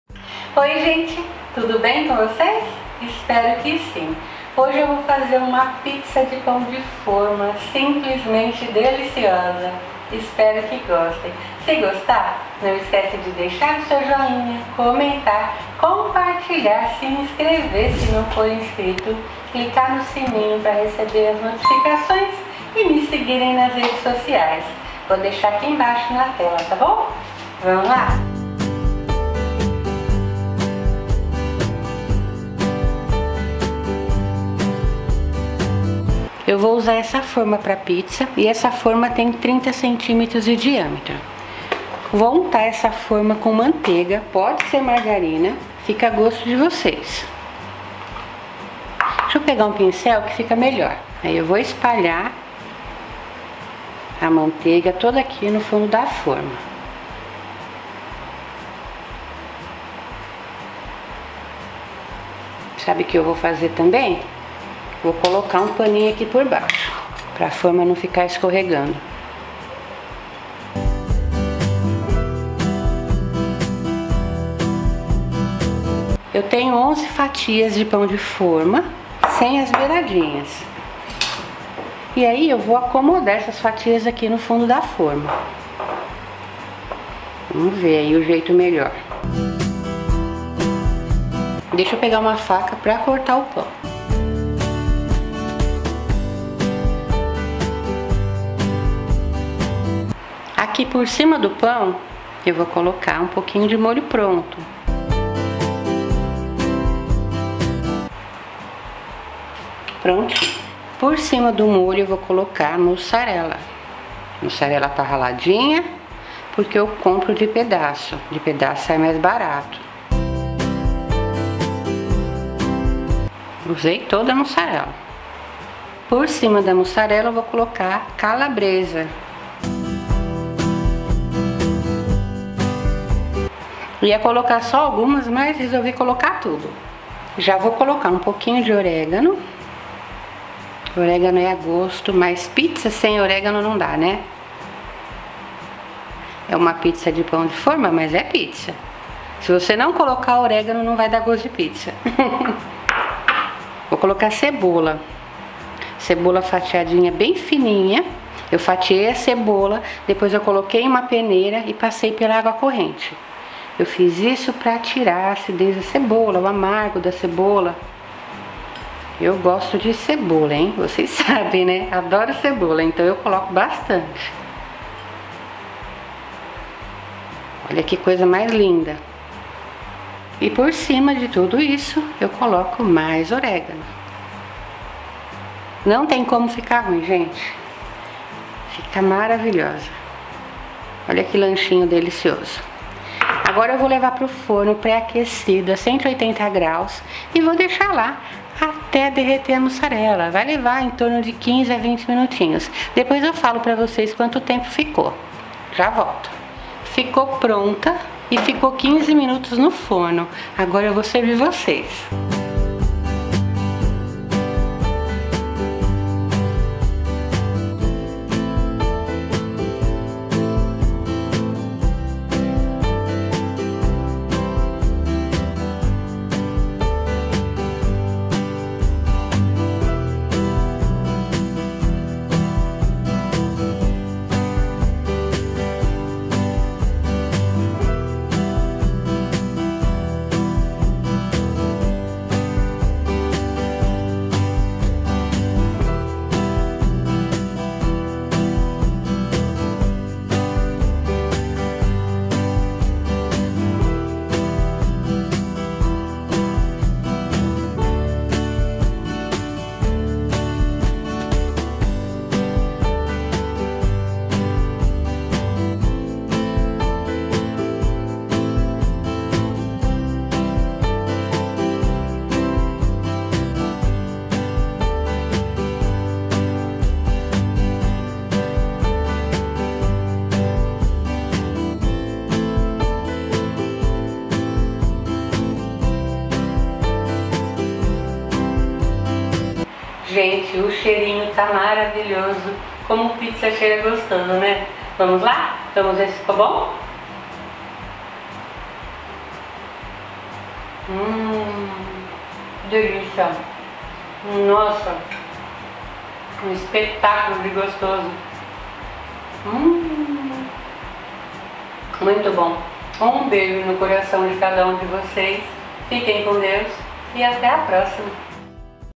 VONTADE DE PIZZA NUNCA MAIS 😱
Olá 😃👋

INGREDIENTES:

Manteiga
Pão de forma
Molho de tomate
Calabresa
Mussarela
Cebola
Orégano